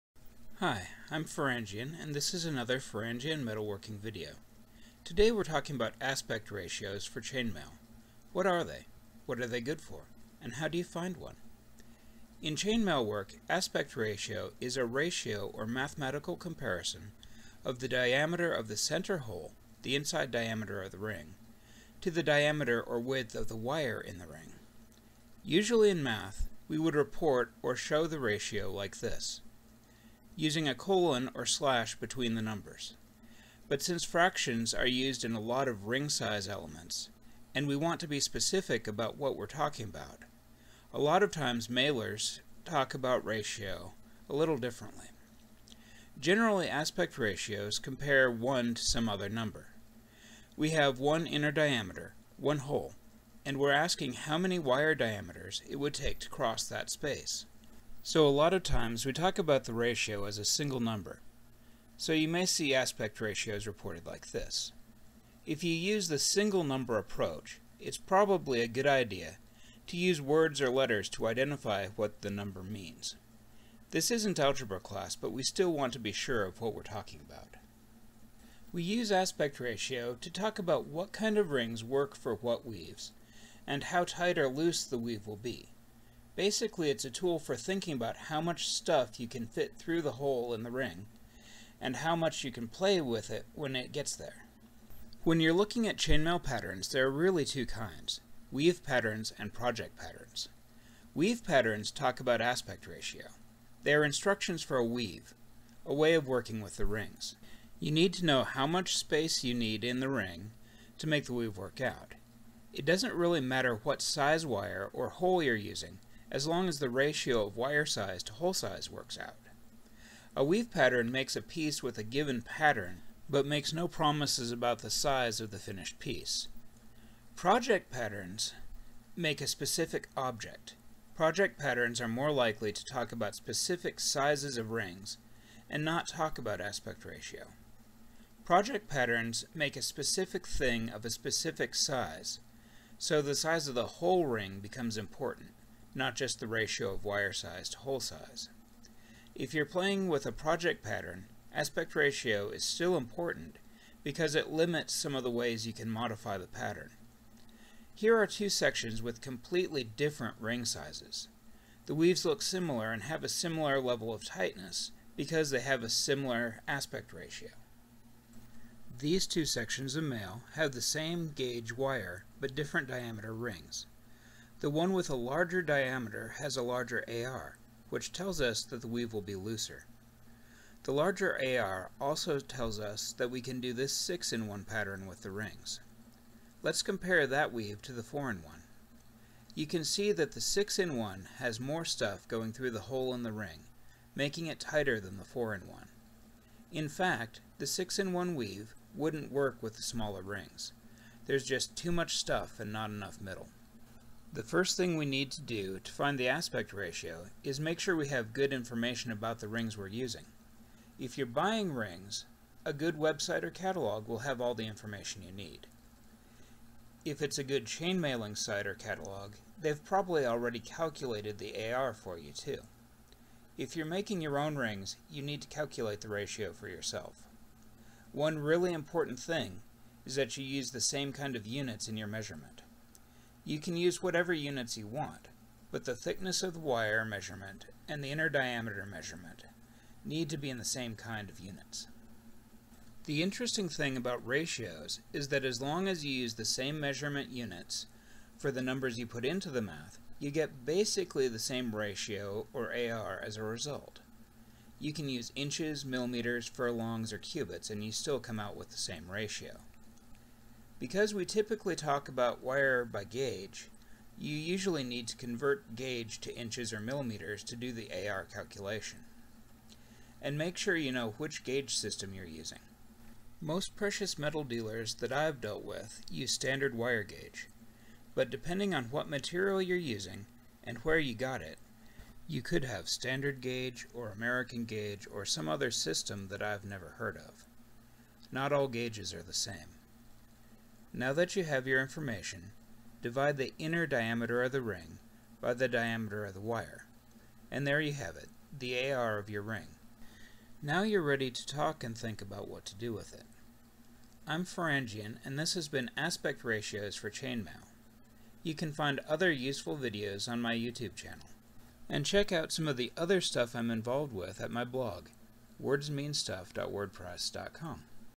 Aspect Ratio for Chainmail
This video covers Aspect Ratios (AR) for use in chainmail (chainmaille) work. It is intended as a supplement for the book Chainmail Bottle Carriers: a fashion statement you can make, and for other art/jewelry projects.
The video illustrates how AR affects mail work, as well as how to use and calculate aspect ratios.
The information in this video (and the others in this collection) are intended to supplement and support the creativity of the viewer. This video is the first in the series to be devoted to how a mathematical concept, the ratio, impacts and interacts with art and the real world.
The video helps the viewer understand how to think about and work with chainmail rings. For information on how to make and use the rings in your own chainmail projects and creations please see the other videos in the collection.
As always the creator welcomes constructive comments/criticisms from the creative community. Let’s create more knowledge together.